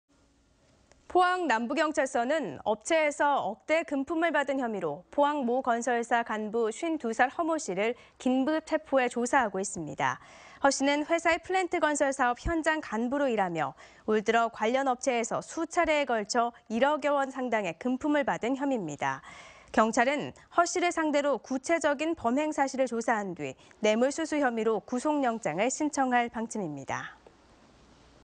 [포항MBC뉴스]건설업체 간부 1억여 원 금품수수 혐의 체포
건설업체 간부 1억여 원 금품수수 혐의 체포